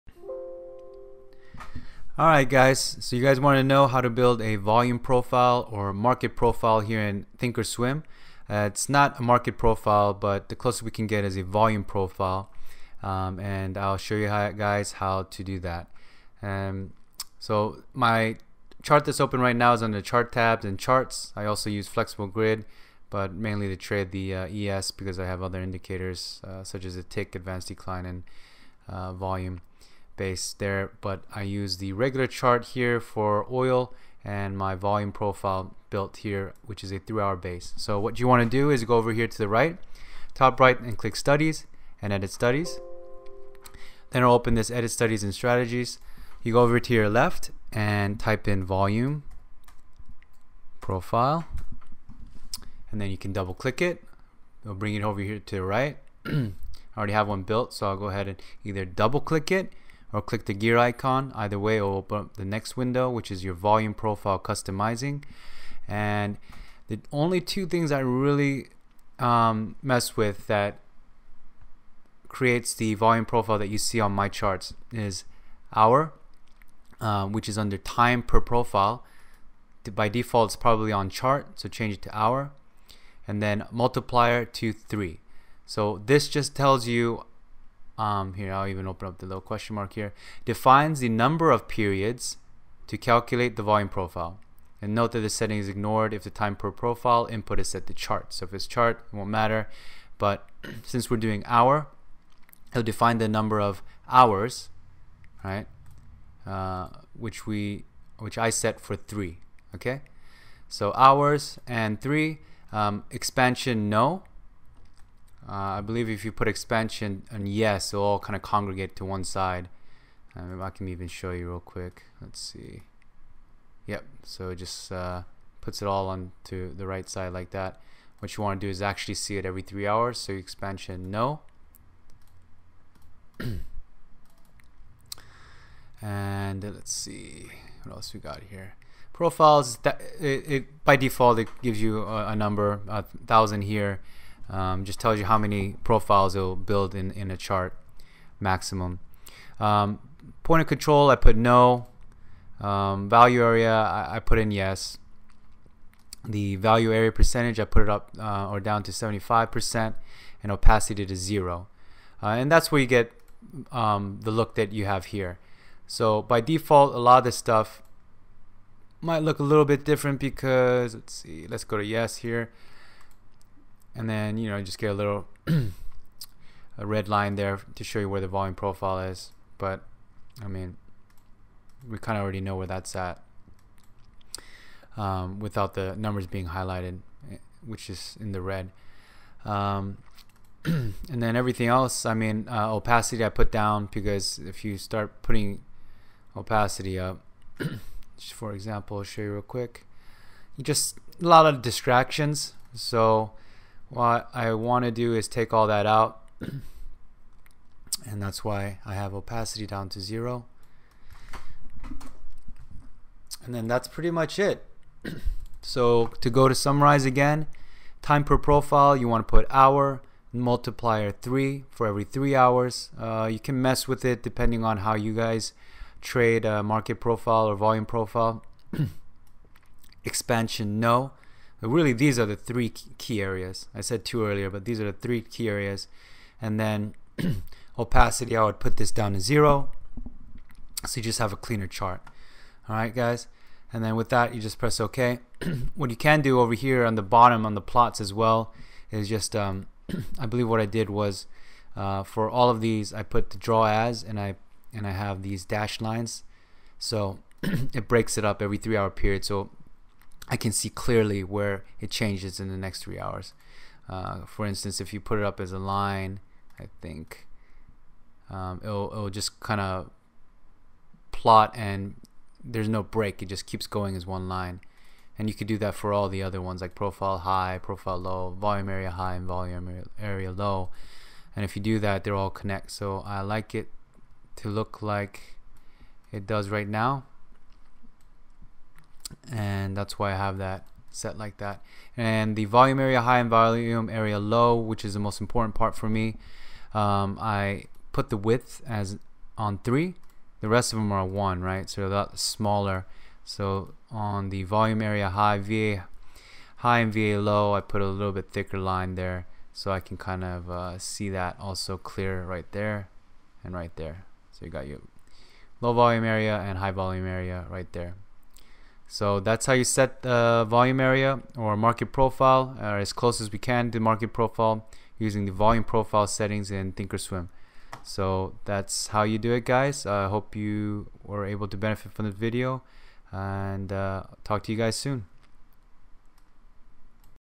How to setup Volume Profile on Thinkorswim
1 October, 2015 - I breakdown the steps to setup volume profile on Thinkorswim. Although it's not exactly market profile it's as close as I've gotten to market profile using the volume profile settings in Thinkorswim.